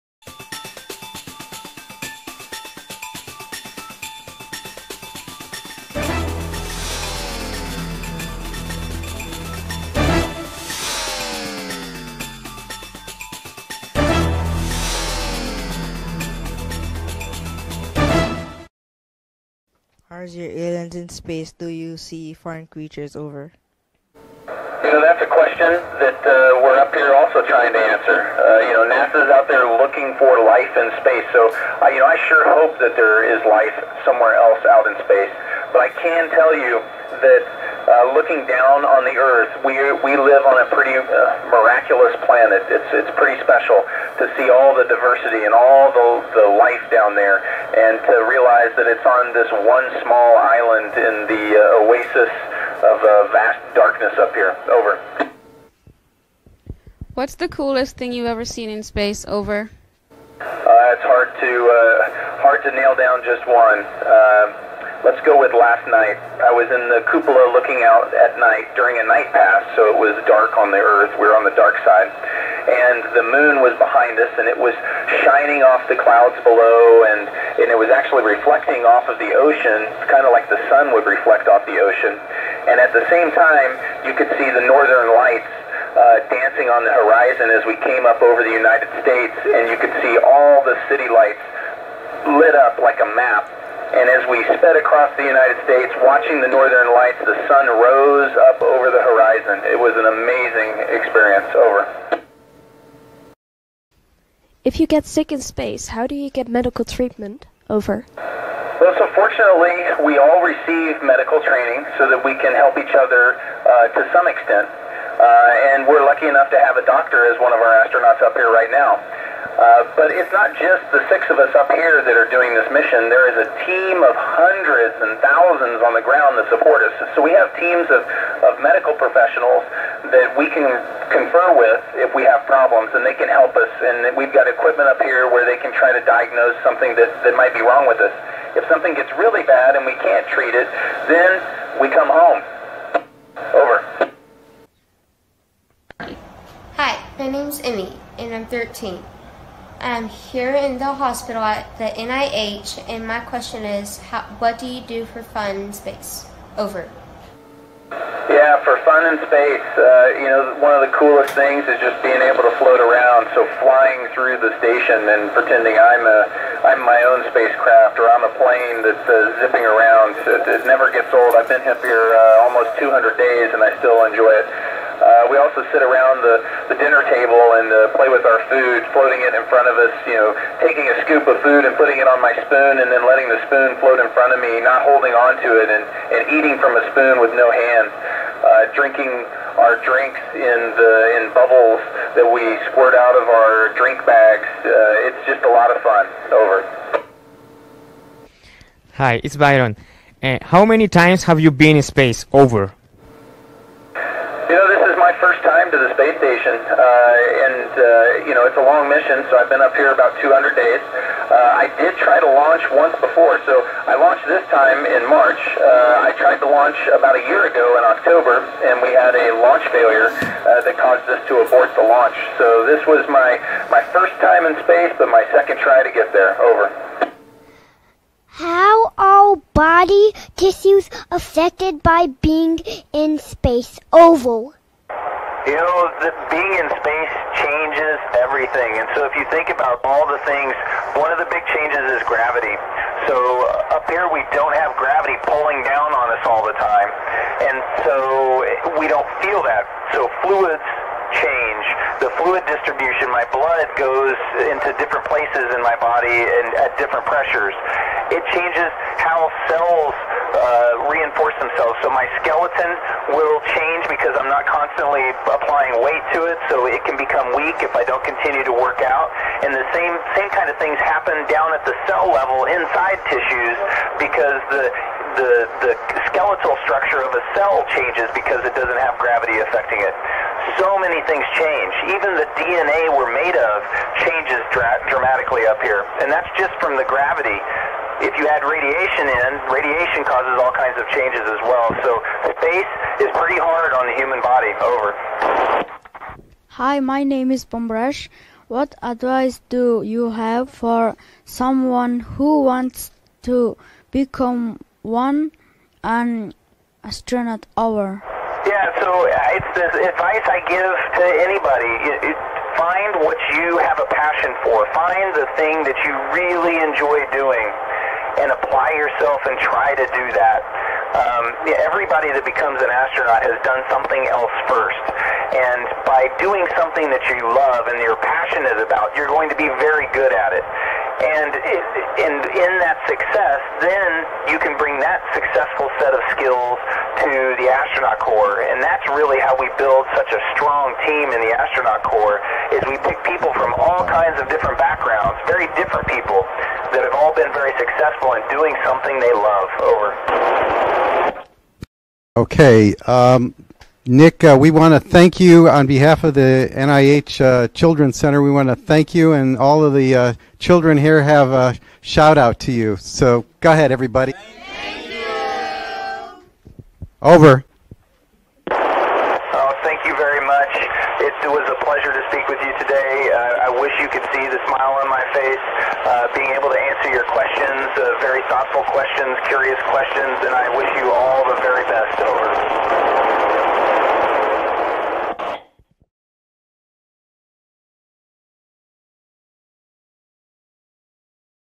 SPACE STATION HAM RADIO CONTACT (edited)
Sept 23 2019. Kids talk to an astronaut using amateur radio.
Edited using the video from the US and my reception in Spain
23 sept de 2019. Niños hablan con un astronauta en la Estación Espacial gracias a la radioafición. Video editado usando las imagenes desde EEUU y mi equipo recibiendo la señal en España